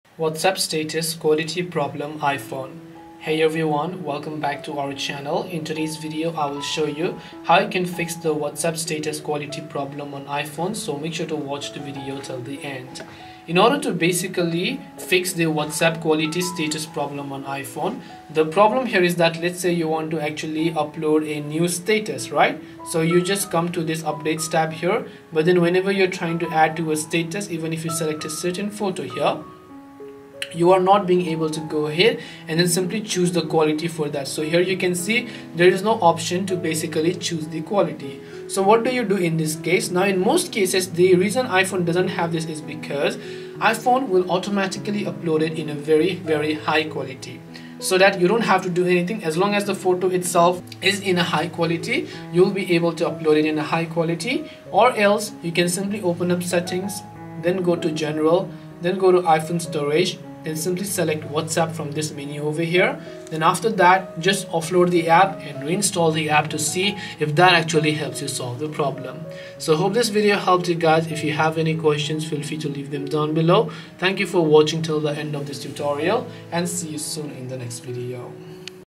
WhatsApp Status Quality Problem iPhone
. This video guides you through an easy step-by-step process to solve the WhatsApp Status quality problem on your iPhone. So make sure to watch this video till the end.

~ Chapters:
0:00 Introduction
0:12 What the Issue Is
0:38 How to Fix It
1:09 Conclusion